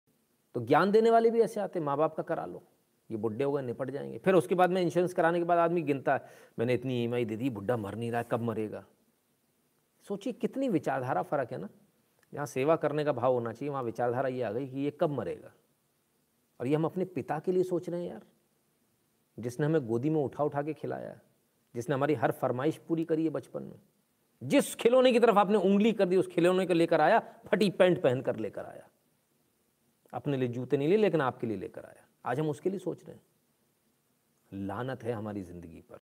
50 Insurance of parents for greed of money
50 Insurance of parents for greed of money
50 Young people sent grandparents in protest in greed of compensation after death
50 Real figure of farmers death in delhi border, cause of death exposed, old men died due to extreme cold and may be corona infection , some by accident by train and tractor while returning